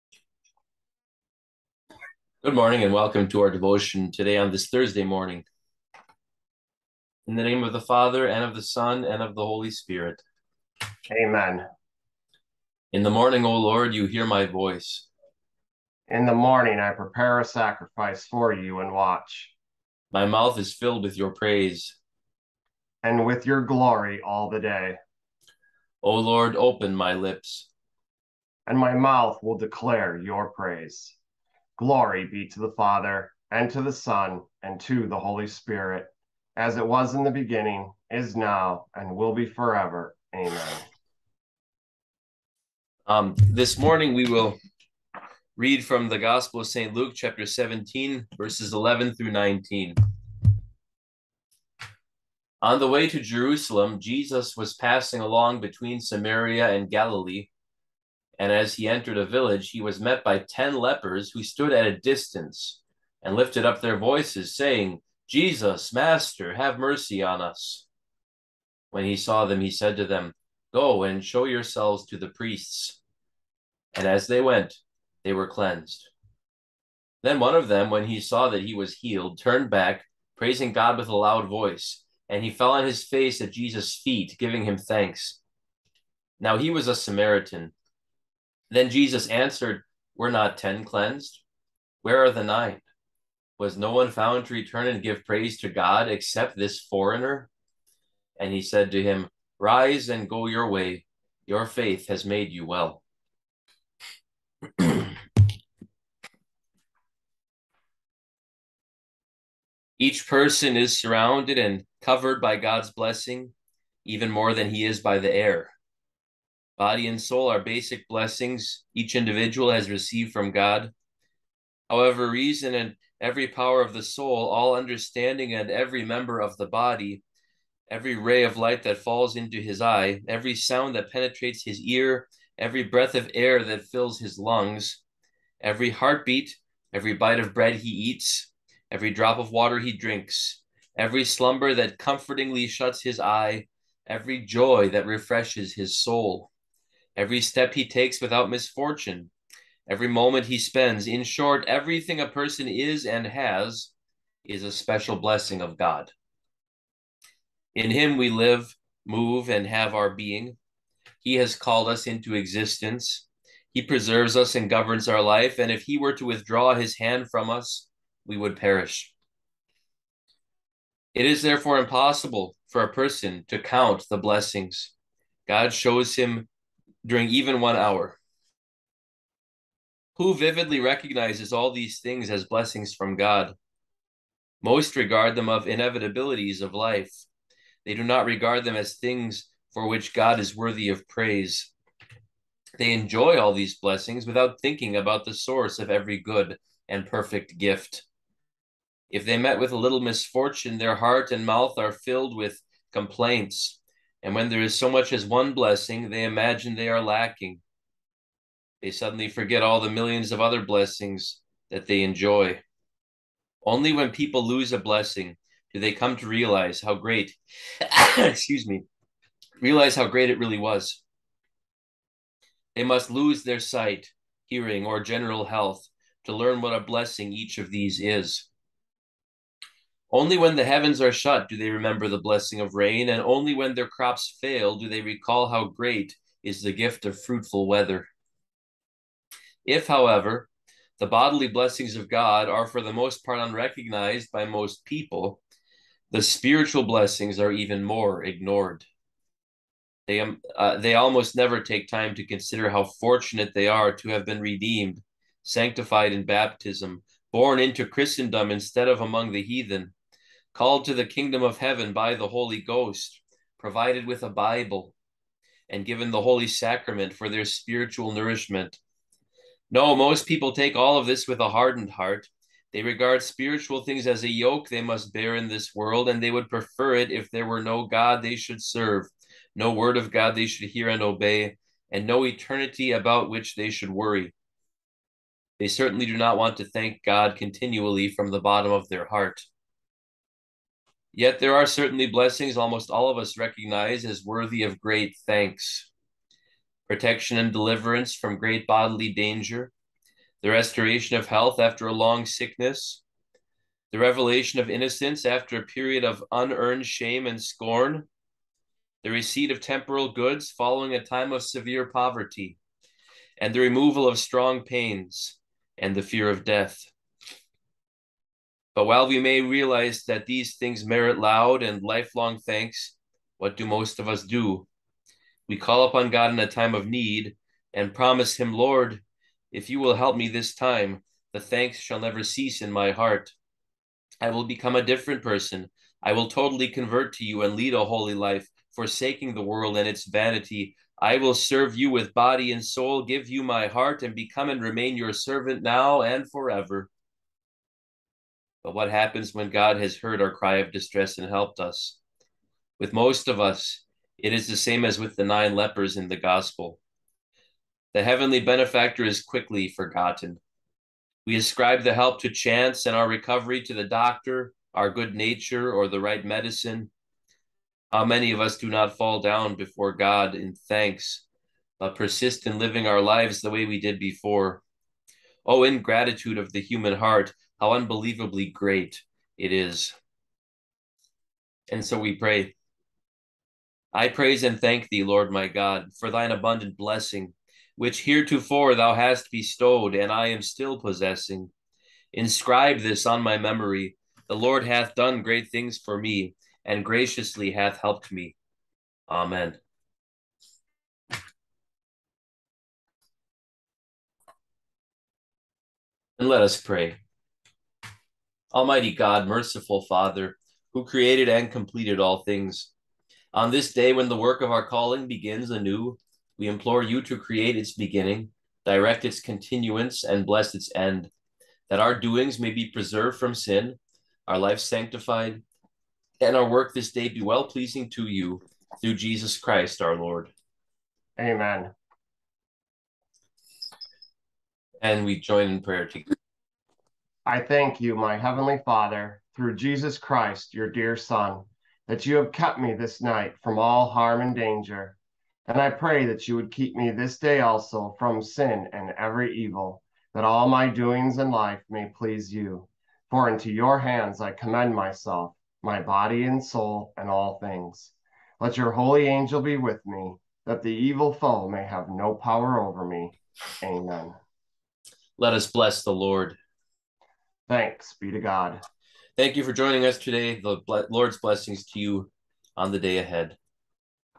Morning Devotion -- "Was no one found to return and give praise to God?"
Luke 17:11-19
God Grant It: Daily Devotions from C.F.W. Walther (p. 701-703)